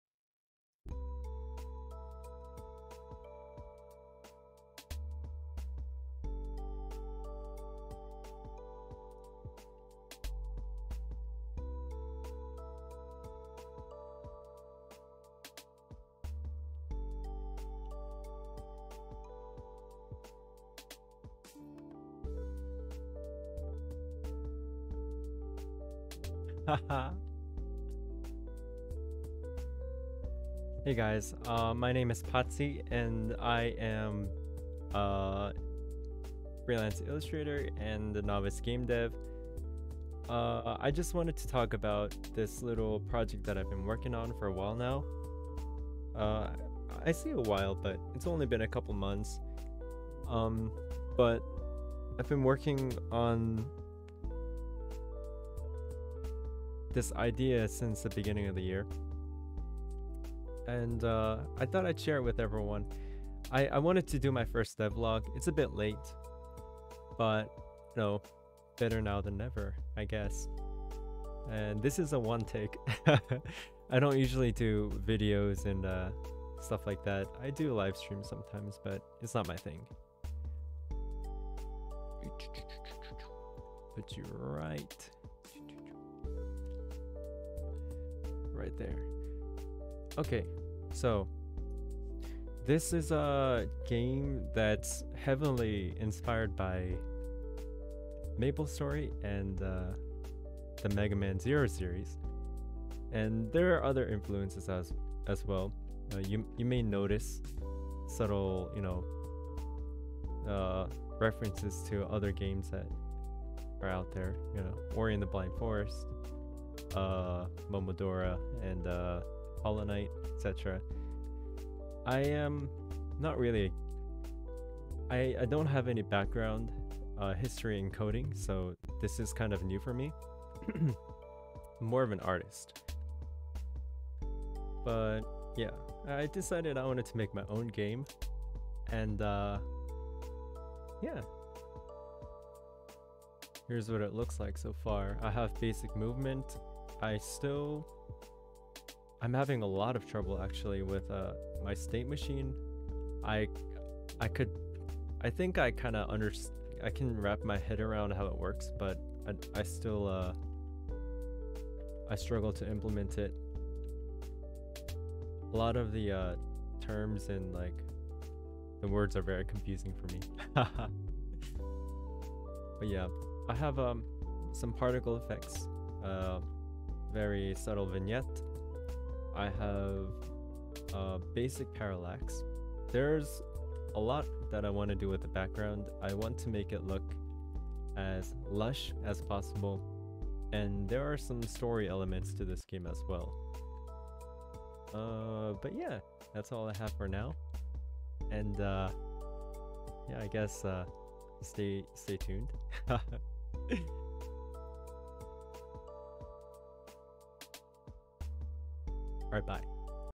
Making My First Game In Godot
I'll try and upload more devlogs as I progress with this project! I'm still learning many things, so bear with me if I forget to post xD